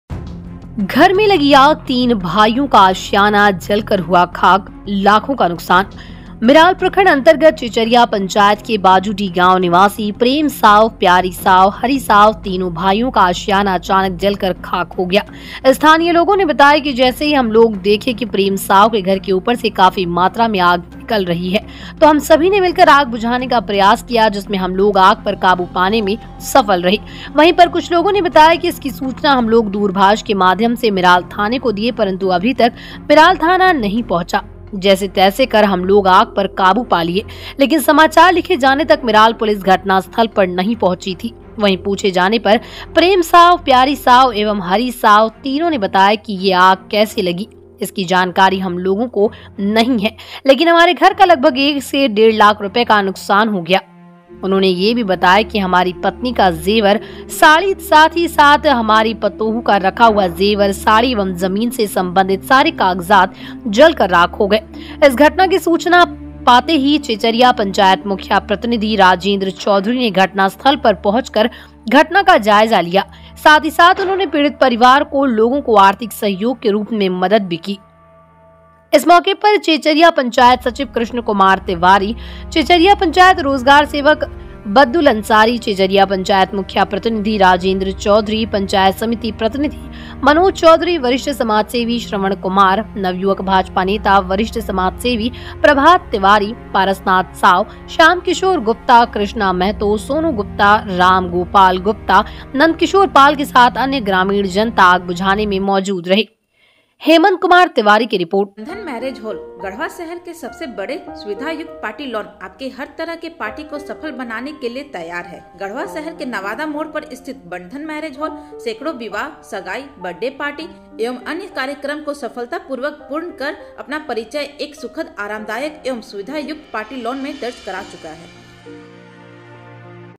घर में लगी आग, तीन भाईयों का आसियाना जल कर हुआ खाक । Garhwa jila news
घर में लगी आग, तीन भाईयों का आसियाना जल कर हुआ खाक, लाखों का नुक़सान

Meral News
Meral ka khabar
Meral garhwa samachar